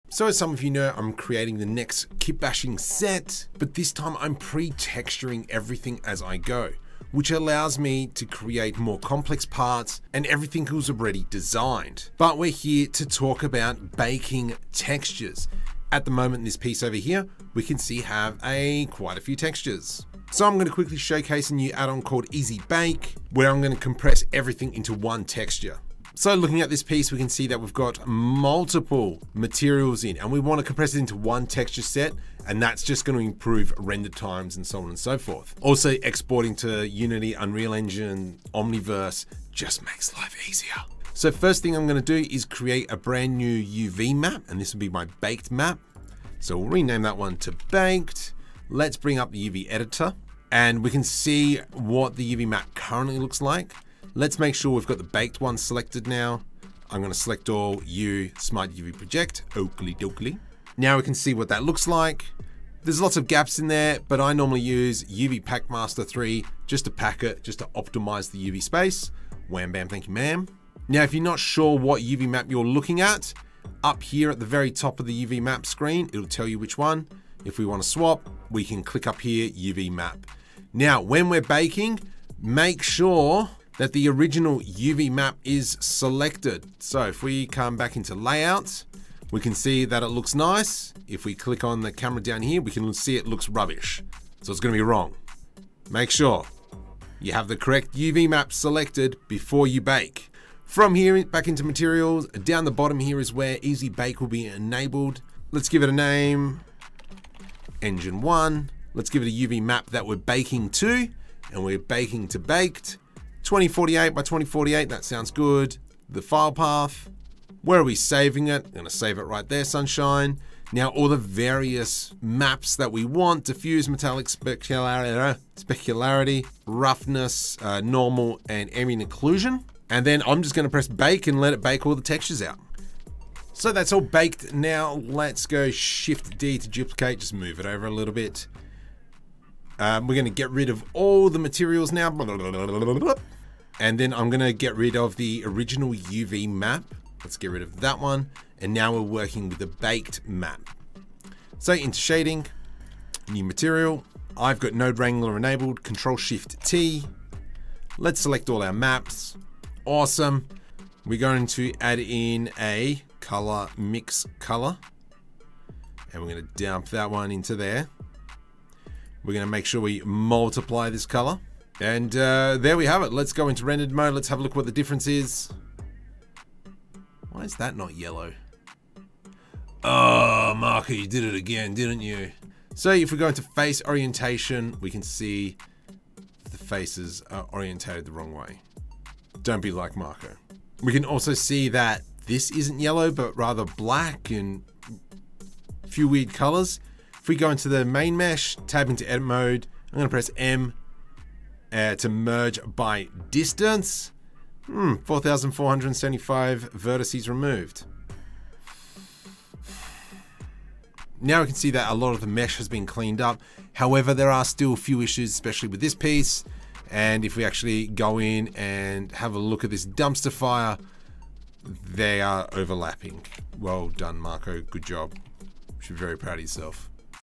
BAKE Texture in BLENDER with just a few clicks - EASY BAKE ADDON
In this video, we're going to learn how to bake texture in Blender with just a few clicks using an easy bake addon. By using this addon, you'll be able to quickly add baked textures to your 3D models without any headache!

If you're looking for an easy way to add baked textures to your 3D models, then this video is for you! By using this addon, you'll be able to quickly and easily add baked textures to your 3D models without any hassle. Plus, this addon is very user friendly and easy to use, so you'll be able to make great textures in no time at all!